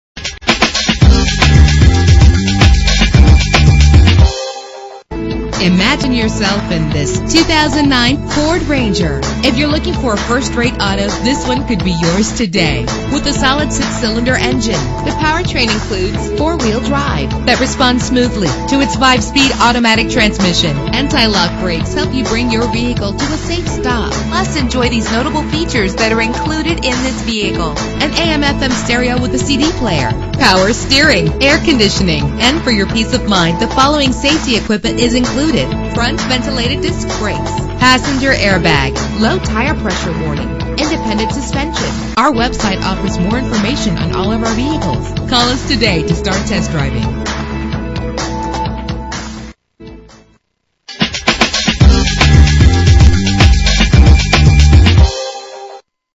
Used 2009 Ford Ranger - Hackensack NJ
Year: 2009
Make: Ford
Model: Ranger
Trim: Sport
Engine: 4.0 liter 6 cylinder 12 valve
Transmission: 5-Speed Automatic
Color: Torch Red Clearcoat
Mileage: 60254
Address:
520 River Street